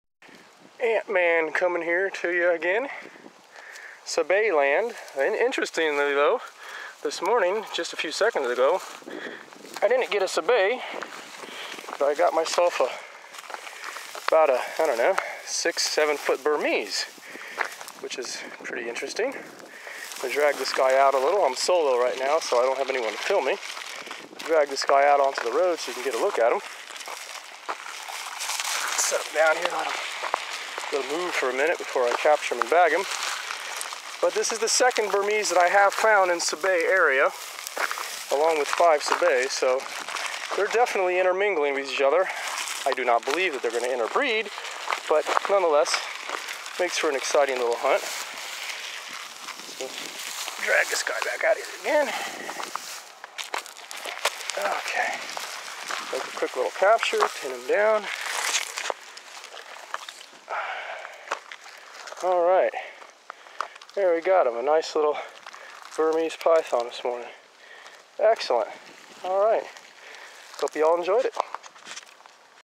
Ant Man catches Burmese Python at Rock Python spot
This snake was found in a location that is better known for producing African Rock Pythons.